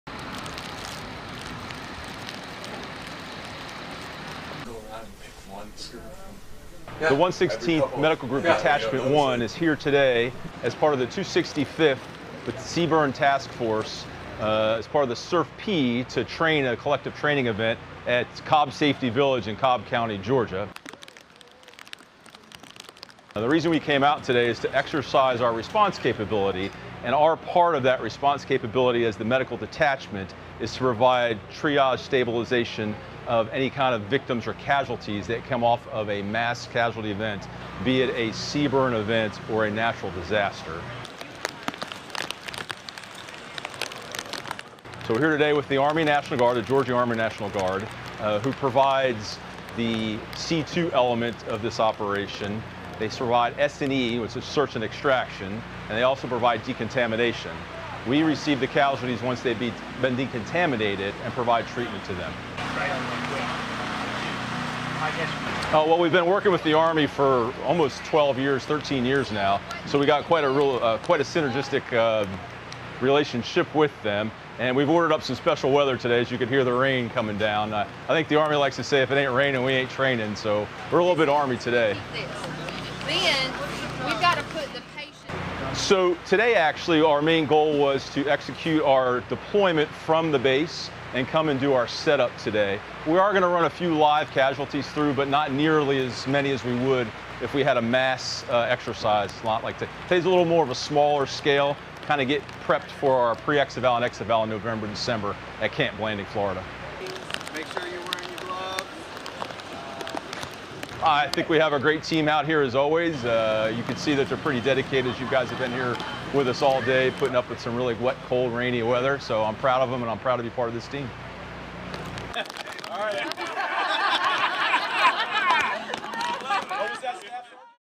Georgia Army and Air National Guard Joint Training Exercise (2019) 🇺🇸
🔴 [Author]:
Video by Airman Josiah Meece

🔴 [Unit]:
116th Air Control Wing Public Affairs

🔴 [Location]:
Marietta, Ga, United States

🔴 [Description]:
U.S. Airmen from the 116th Medical Group, Detachment 1, Georgia Air National Guard, and Soldiers from the 265th Chemical Battalion, Georgia National Guard, conduct a joint training exercise at the Cobb Safety Village, Marietta, Ga., Oct. 19, 2019. The training event was conducted to exercise their response capabilities to natural or man-made disasters as a part of the Homeland Response Force. Should a disaster occur, the Army and Air National Guard Units work together assisting civil authorities to provide search and extraction, decontamination in case of a chemical, biological, radiological, or nuclear event and medical triage to care for patients on the scene. (U.S. Air National Guard video by Airman Josiah Meece)

🔴 [Tags]:
Medical, NGB, Robins AFB, Joint Exercise, Doctors, Homeland Response Force, Georgia ANG, 116 ACW

🔴 [VIRIN]:
191019-Z-XU318-1001